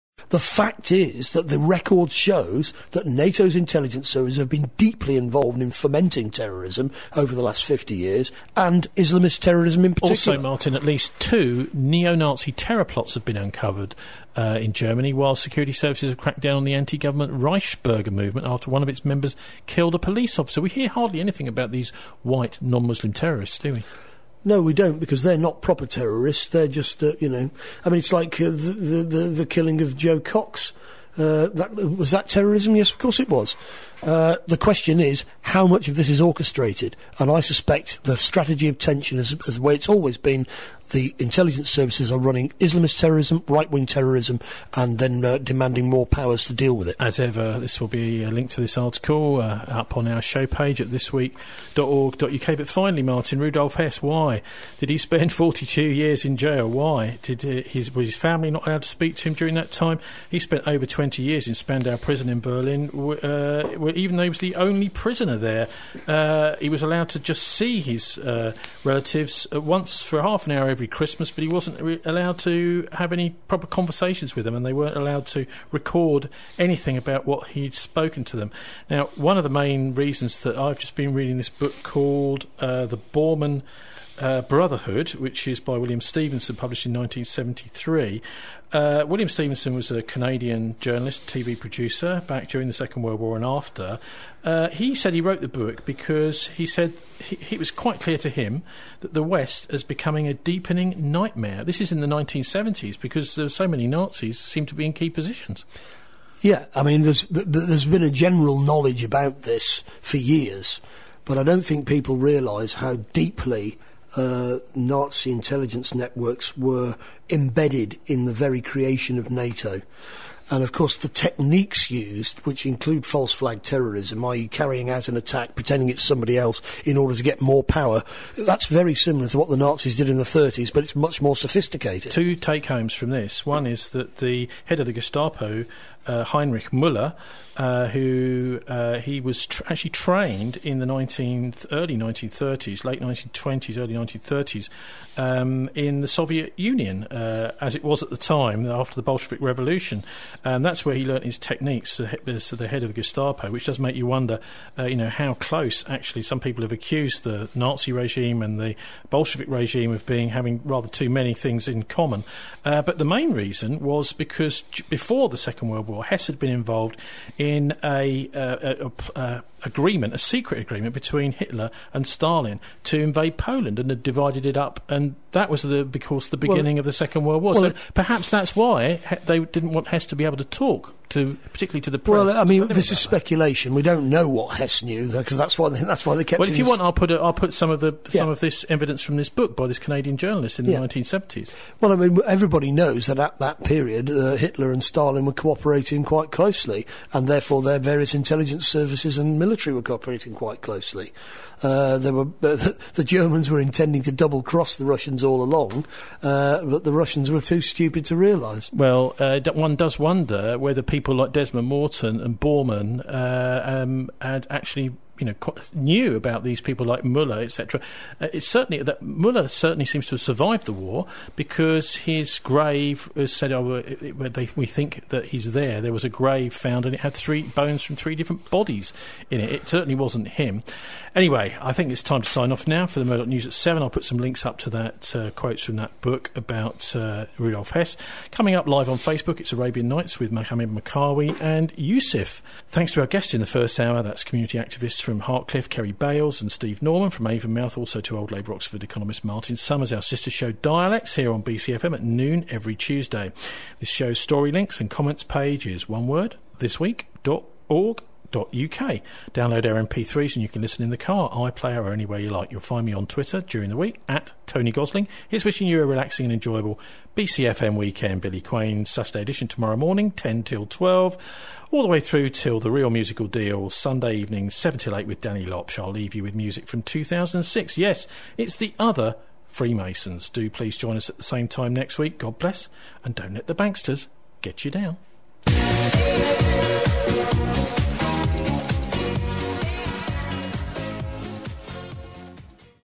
Rudolf Hess: 42 years alone in Spandau jail, what did Hitler's deputy know?
Bolshevik = Nazi?
Gestapo chief Heinrich Muller was trained by the
Bolshevik Cheka secret police around 1930 in the
Soviet Union
Hitler's deputy Rudolf Hess was imprisoned in Spandau
Prison for 42 years, the rest of his life after WWII,
dying in 1987 aged 93 – why?
Nazis working closely with Soviet Union dividing up
Eastern Europe before WW2 and preparing for war
together

The Bormann Brotherhood, William Stevenson (1973)